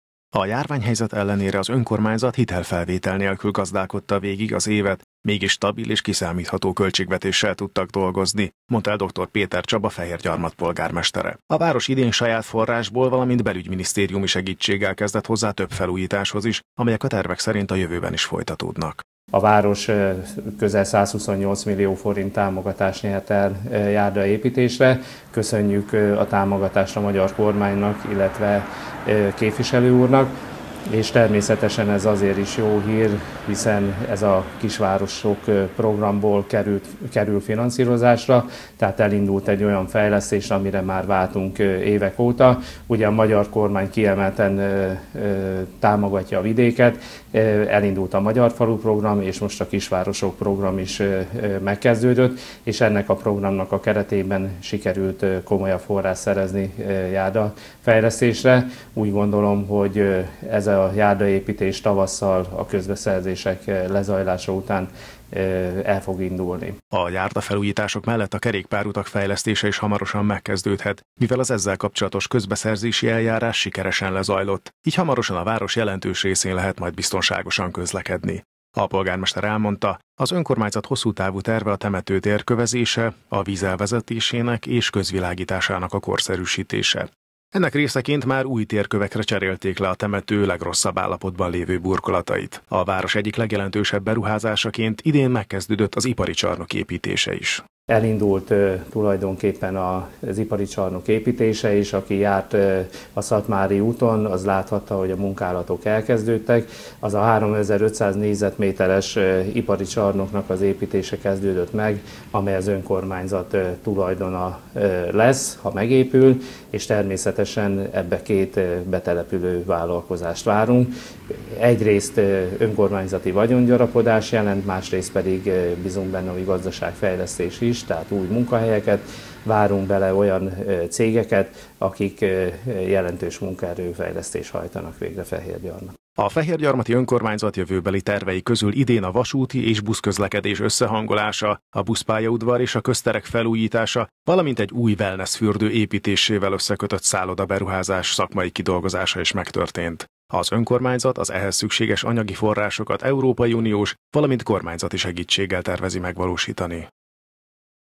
Gazdasági fejlődést várnak
Gazdasági fejlődést és új munkahelyeket várnak Fehérgyarmaton attól az épülő ipari csarnoktól, amelybe betelepülő vállalkozásokat várnak. Az önkormányzat tavaly járdákat újított fel és korszerű közvilágítást telepített. Az évet pedig stabil, kiszámítható költségvetéssel zárták Fehérgyarmaton.